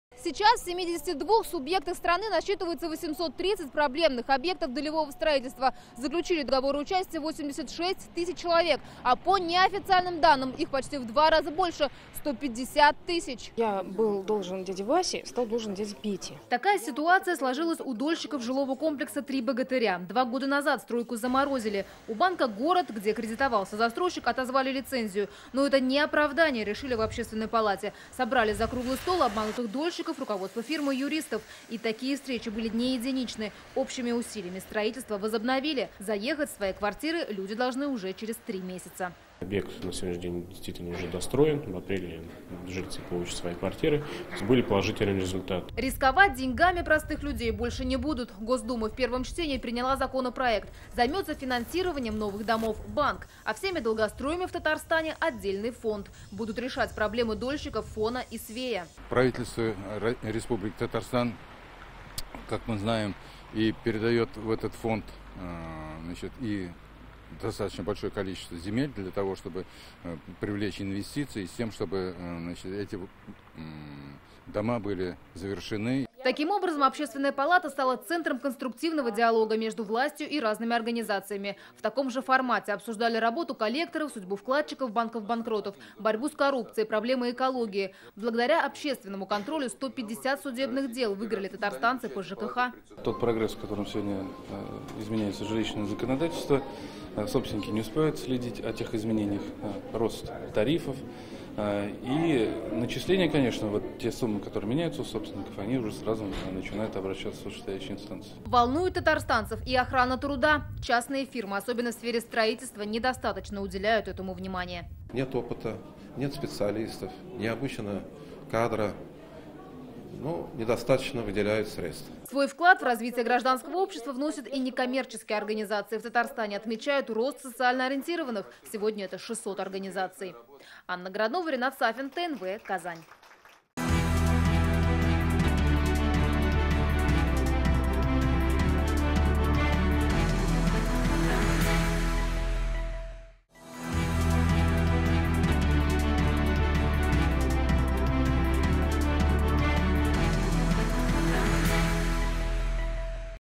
Контроль за строительными компаниями ужесточат, а для их владельцев введут штрафы - ТНВ
Это для тех, кто обманул дольщиков. Госдума в первом чтении приняла законопроект, который предусматривает госконтроль в области долевого строительства. В Татарстане с прошлого года по поручению Рустама Минниханова создана специальная комиссия по защите прав дольщиков.

Координаты:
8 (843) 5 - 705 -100

Таяну ноктасы. В эфире: пн, вт, ср, чт - 19:00

7 дней. В эфире: вс - 19:30, 21:30